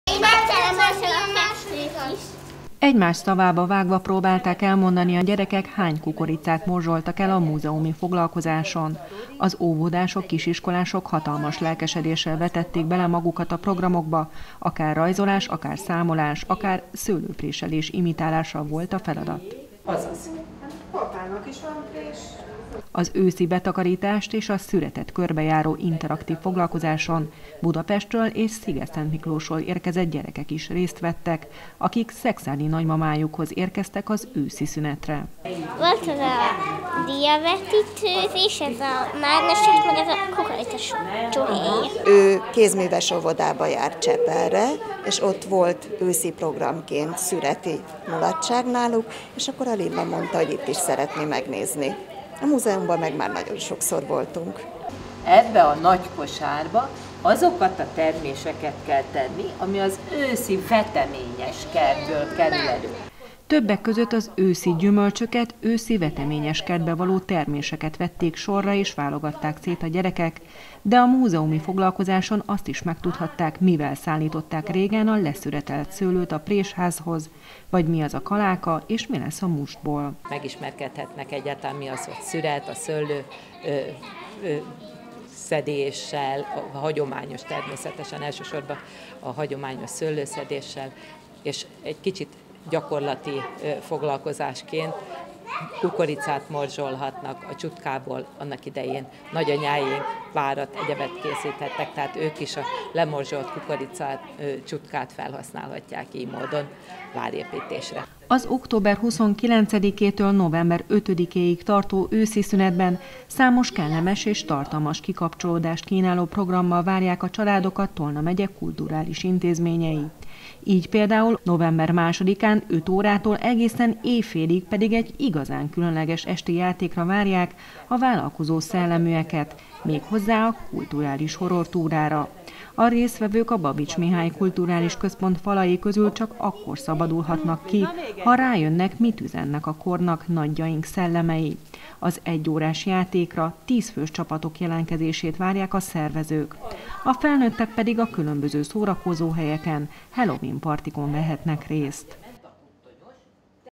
Öszi szünet - programok 20181031
Október 29-től egészen november 5-éig tart az őszi szünet. Ilyenkor a szülők, nagyszülők igyekeznek színes programokkal megkínálni a gyerekeket. Szekszárdon több lehetőség közül választhatnak, például a múzeumban őszi betakarítást körbejáró interaktív foglalkozáson vehetnek részt a gyerekek, a babits mihály kulturális központban pedig kulturális horrortúrán találhatják ki, mit üzennek a kornak nagyjaink szellemei.